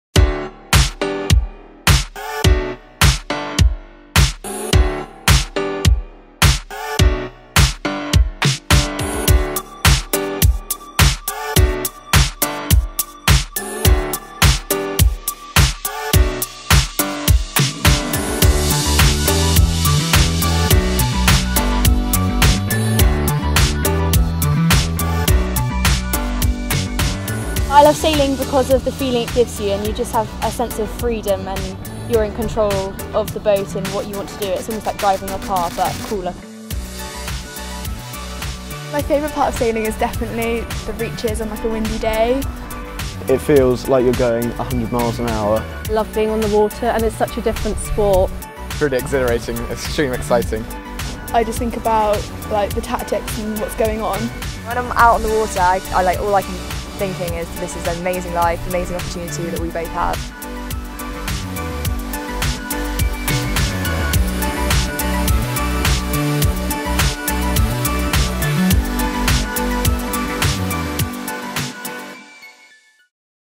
Neilson GBR Cadet World Team
Neilson are delighted to be supporting the GBR Cadet World Team on their quest for success at the World Championships in Buenos Aires, Argentina.  Here we capture the team in action during a training weekend at the Weymouth & Portland National Sailing Academy.